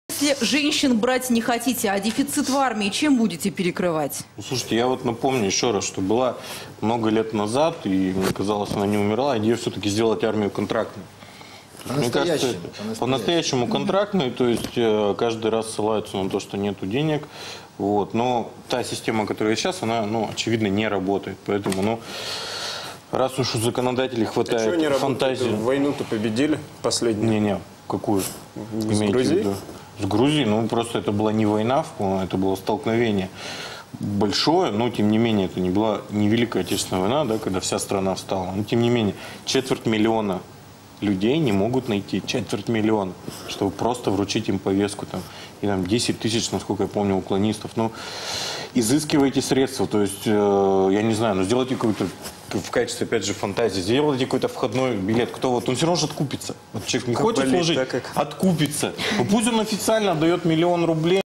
Депутат предложил официально откупаться от армии
Депутат считает, что сейчас очень многие молодые люди призывного возраста просто-напросто могут откупиться от службы. Так вот надо внедрить такую систему, чтобы  юноша мог официально за деньги отказаться от пребывания в российской армии.
«Сделайте какой-нибудь входной билет. Вот человек не хочет служить -- откупится. Ну, пусть он официально отдает миллион рублей», - пояснил депутат-справедливоросс Андрей Жуковский.